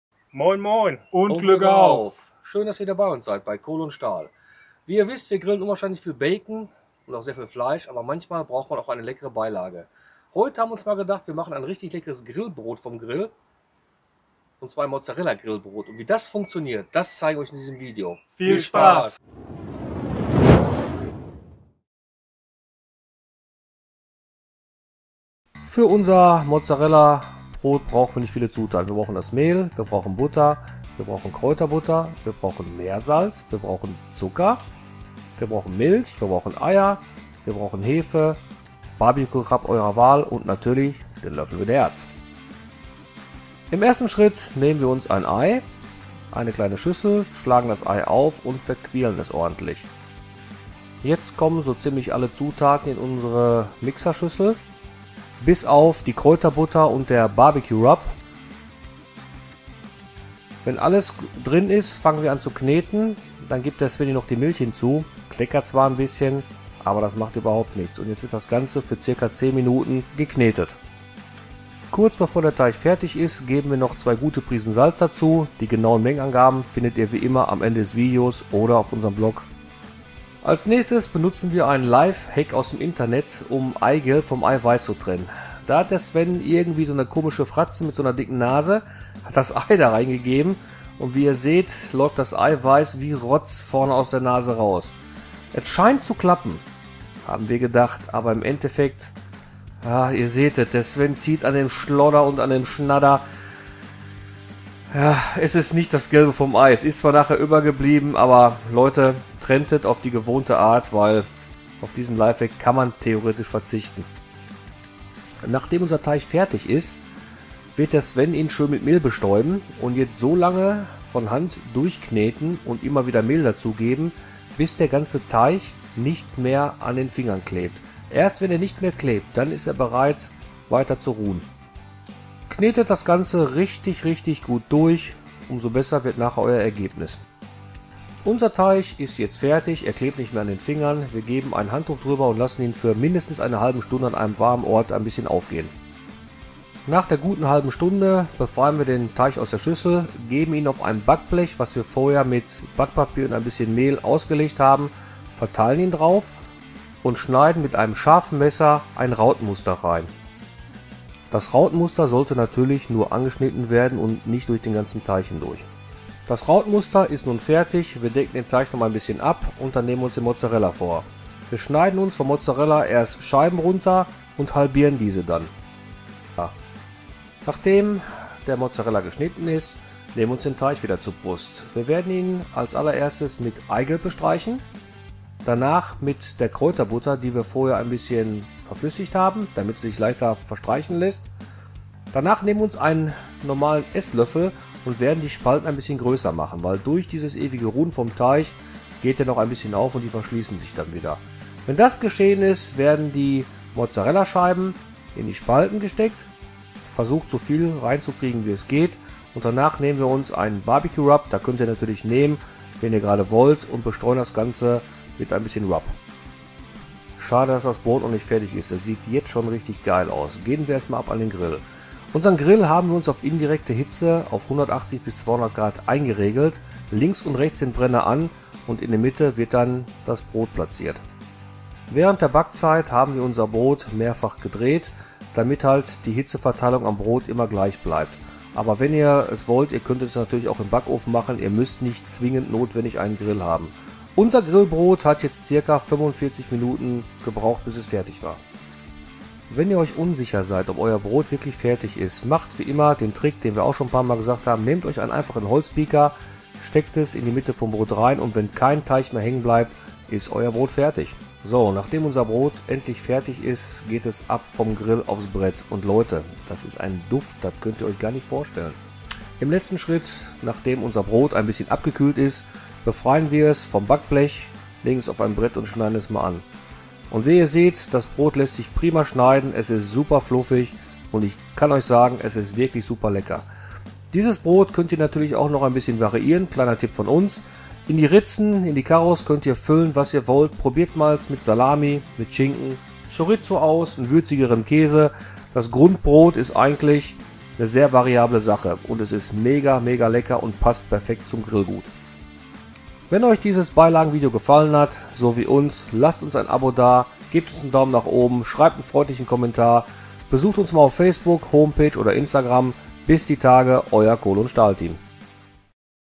Mozzarellabrot - K&S Folge - 029
Da es nicht immer nur Fleisch sein muss zeigen wir heute ein leckeres Grillbrot. Schön cheesy und pikant!